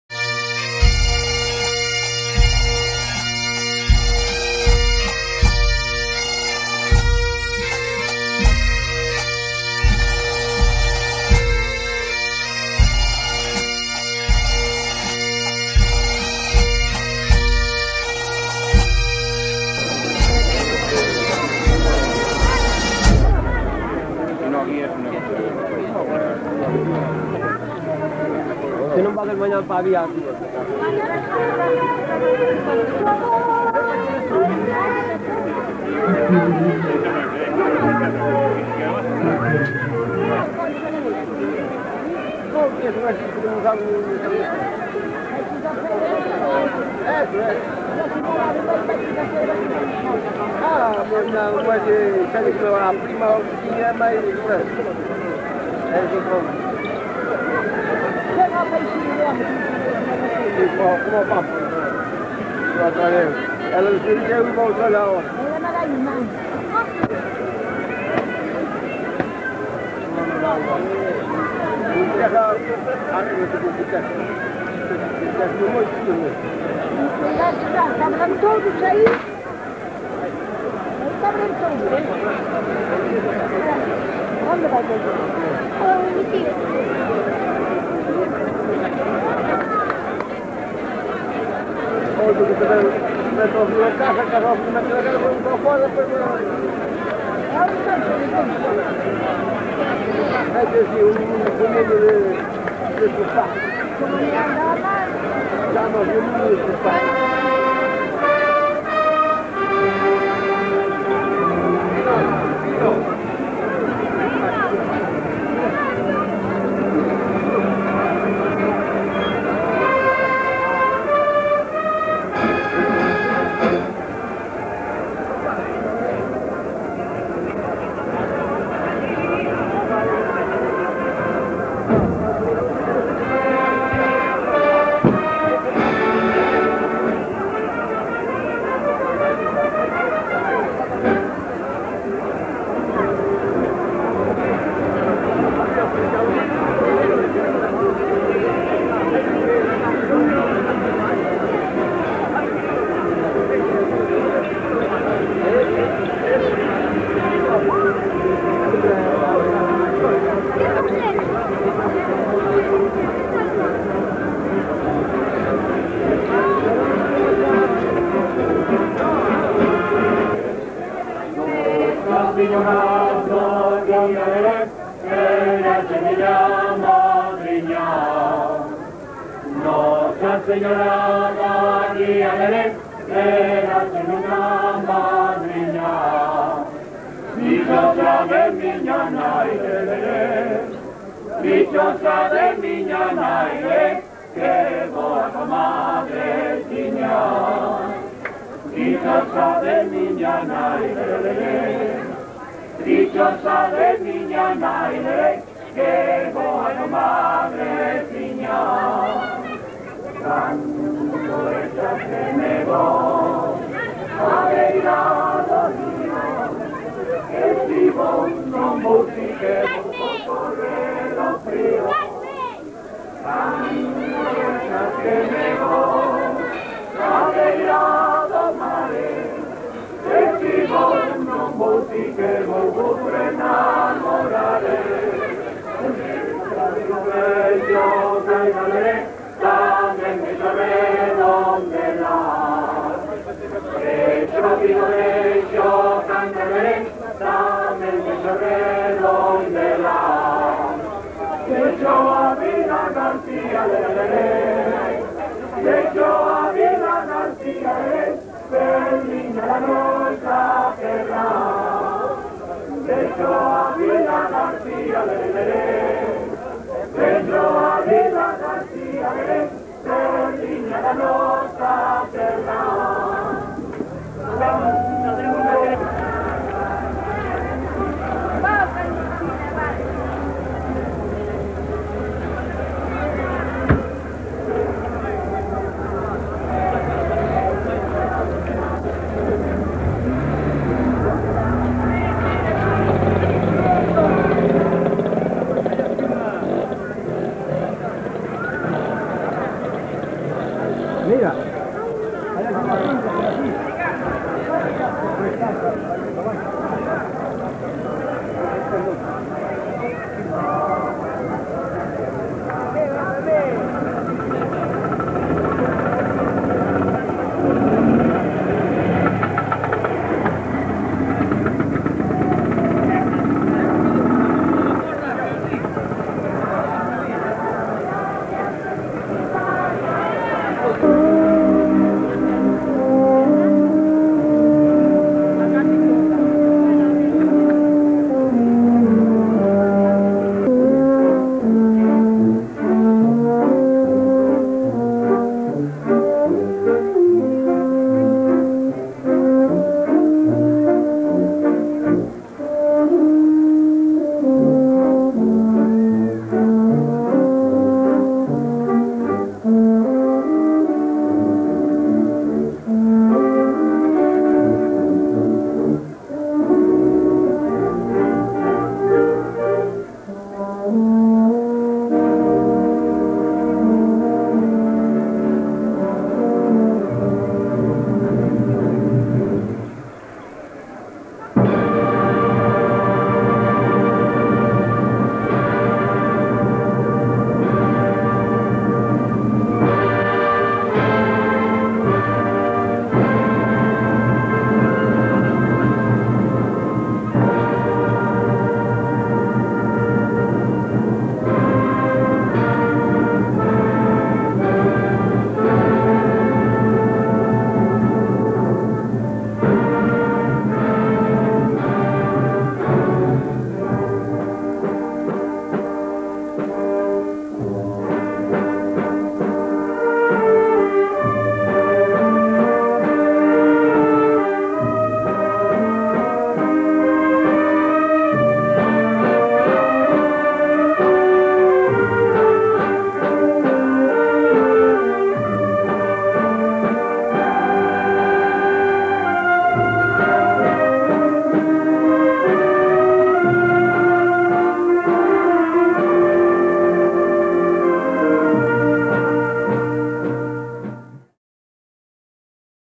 Festas de Santa Pracida 1984. Primeira Parte
O campo da festa, coral no bar O Relojero e actuacion da banda